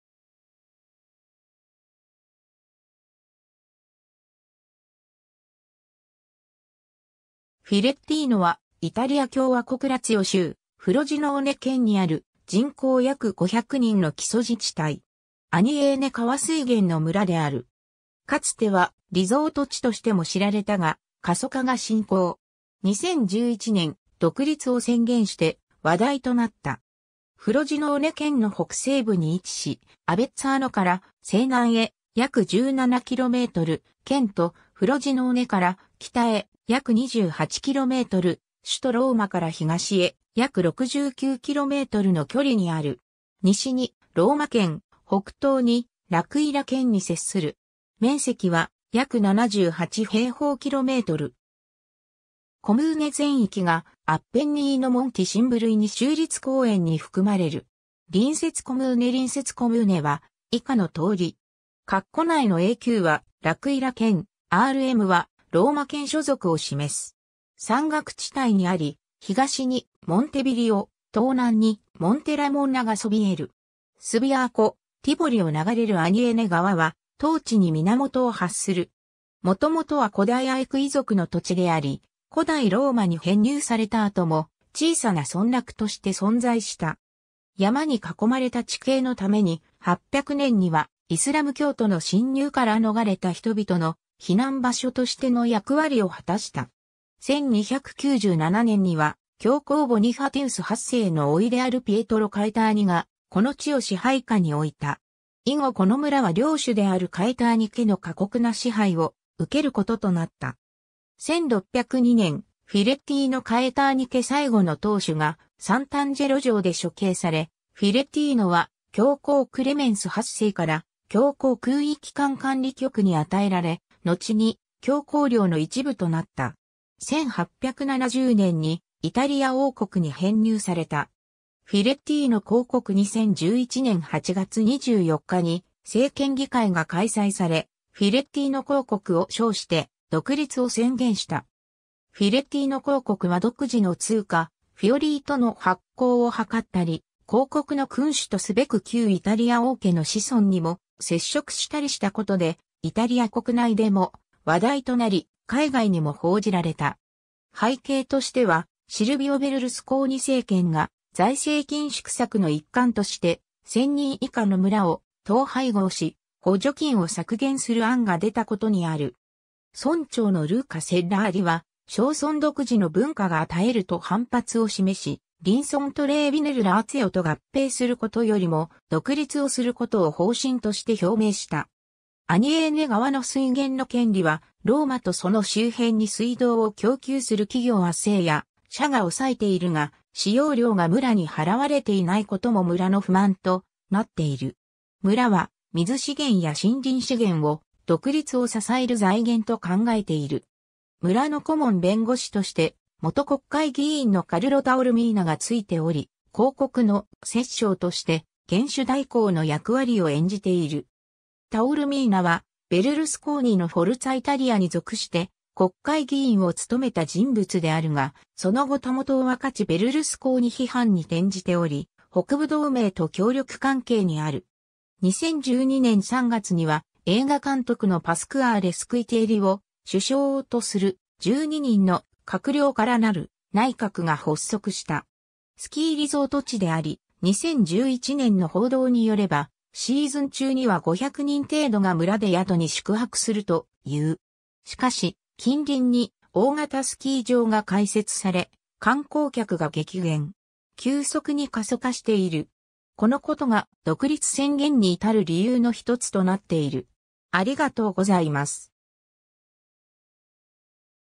フィレッティーノ
フィレッティーノ（イタリア語: Filettino）は、イタリア共和国ラツィオ州フロジノーネ県にある、人口約500人の基礎自治体（コムーネ）。
 アニエーネ川（テヴェレ川水系）水源の村である。
かつてはリゾート地としても知られたが過疎化が進行。
2011年、「独立」を宣言して話題となった。
 フロジノーネ県の北西部に位置し、アヴェッツァーノから西南へ約17km、県都フロジノーネから北へ約28km、首都ローマから東へ約69kmの距離にある。
西にローマ県、北東にラクイラ県（アブルッツォ州）に接する。
 面積は約78平方km。
コムーネ全域がアッペンニーノ・モンティ・シンブルイニ州立公園 (it:Parco naturale regionale dell'Appennino - Monti Simbruini) に含まれる。
 隣接コムーネ 隣接コムーネは以下の通り。
括弧内のAQはラクイラ県（アブルッツォ州）、RMはローマ県所属を示す。
 山岳地帯にあり、東にモンテヴィリオ（Monte Viglio）、東南にモンテラモンナ（Monte la Monna）が聳える。
 スビアーコ、ティボリを流れるアニエーネ川（テヴェレ川水系）は、当地に源を発する。
 もともとは古代アエクイ族の土地であり、古代ローマに編入されたあとも小さな村落として存在した。
山に囲まれた地形のために、800年にはイスラム教徒の侵入から逃れた人々の避難場所としての役割を果たした。
 1297年には、教皇ボニファティウス8世の甥であるピエトロ・カエターニがこの地を支配下に置いた。
以後この村は領主であるカエターニ家の過酷な支配を受けることとなった。
1602年、フィレッティーノ・カエターニ家最後の当主がサンタンジェロ城で処刑され、フィレッティーノは教皇クレメンス8世から教皇空位期間管理局 (Apostolic Camera) に与えられ、のちに教皇領の一部となった。
1870年にイタリア王国に編入された。
 「フィレッティーノ公国」 2011年8月24日に「制憲議会」(Assemblea Costituente) が開催され、「フィレッティーノ公国」（Principato di Filettino）を称して「独立」を宣言した 。
「フィレッティーノ公国」は独自の通貨「フィオリート」の発行を図ったり、「公国」の君主とすべく旧イタリア王家（サヴォイア家）の子孫にも接触したりしたことで、イタリア国内でも話題となり、海外にも報じられた。
 背景としては、シルヴィオ・ベルルスコーニ政権が財政緊縮策の一環として、1000人以下の村（コムーネ）を統廃合し補助金を削減する案が出たことにある。
村長（当時）のルーカ・セッラーリ (Luca Sellari) は「小村独自の文化が絶える」と反発を示し、隣村トレーヴィ・ネル・ラーツィオと合併することよりも、「独立」をすることを方針として表明した。
アニエーネ川の水源の権利は、ローマとその周辺に水道を供給する企業 ACEA社 が押さえているが、使用料が村に払われていないことも村の不満となっている。
村は水資源や森林資源を「独立」を支える財源と考えている。
 村の顧問弁護士として、元国会議員のカルロ・タオルミーナ (it:Carlo Taormina) がついており、「公国」の「摂政」(principe reggente) として元首代行の役割を演じている。
タオルミーナは、ベルルスコーニのフォルツァ・イタリアに属して国会議員を務めた（2001年には短期間ながら内務省の政務次官を務めた）人物であるが、その後袂を分かちベルルスコーニ批判に転じており、北部同盟と協力関係にある。
 2012年3月には、映画監督のパスクアーレ・スクイティエリ (it:Pasquale Squitieri) を「首相」とする、12人の「閣僚」からなる「内閣」が発足した。
 スキーリゾート地であり、2011年の報道によれば、シーズン中には500人程度が村で宿に宿泊するという。
しかし、近隣に大型スキー場が開設され、観光客が激減。
急速に過疎化している。
このことが「独立」宣言にいたる理由の一つとなっている。